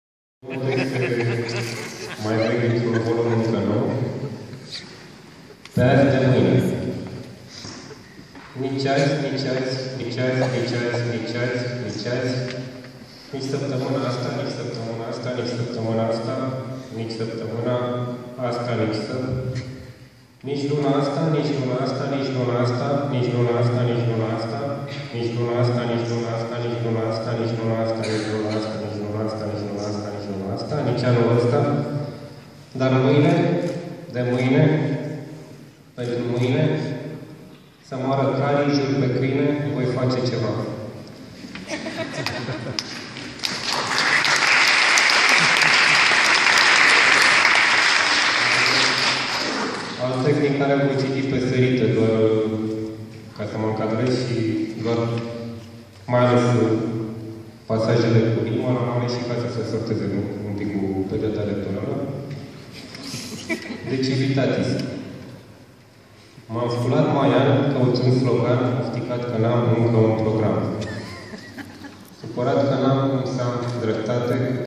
Maraton de poezie - Sorin Gherguţ
Sorin Gherguţ la Maratonul de poezie si jazz, ediţia a VIII-a, în cadrul Festivalului Internaţional de Poezie Bucureşti (FIPB)
21 mai 2016